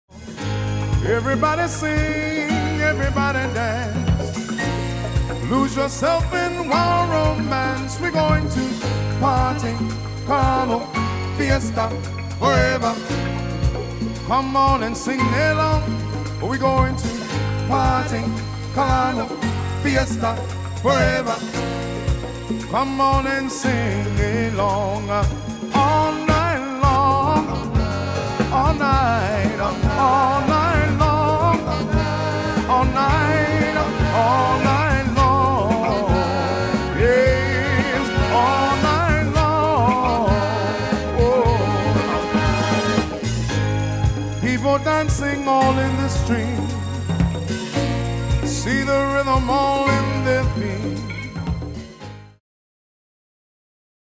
DeasGuyzOrchestra
The Jazz For All Ages 2021 Music Festival in Hilton Head Island welcomes back The Deas Guyz Orchestra LIVE at The HH Beach and Tennis Resort Ballroom on October 28th! Tickets are available at jazzforallages.com